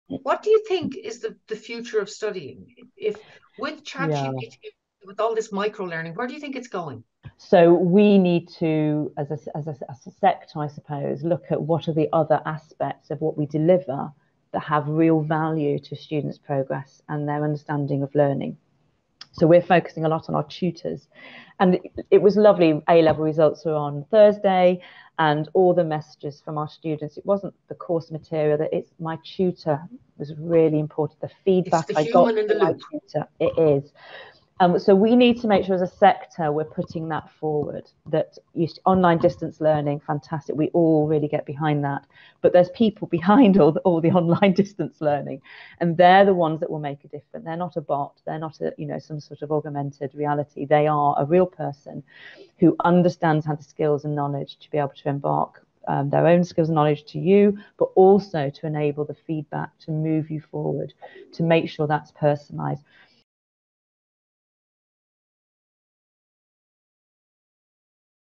EADL Meet the Members - Ellen Gunning, IAPR - Esther Chesterman, National Extension College
An EADL "Meet the Members".  Esther Chesterman of National Extension College interview by Ellen Gunning, Irish Academy of Public Relations, on the future of learning and the importance of human interaction in the world of AI.